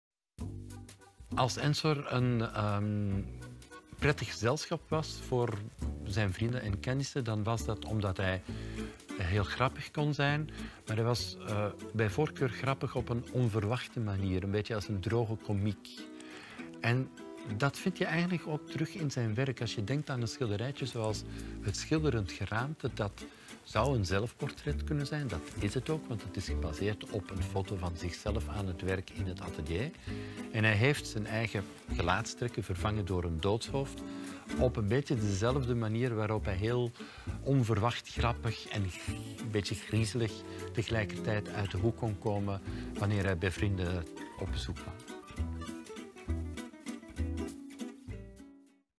James Ensor in vogelvlucht - Ensor de Komiek
Ensor had veel droge humor en was grappig op een onverwachte manier. Dat maakte hem prettig gezelschap voor zijn vrienden én dat zag je in zijn werk. Ensor-kenner Herwig Todts vertelt er ons graag meer over.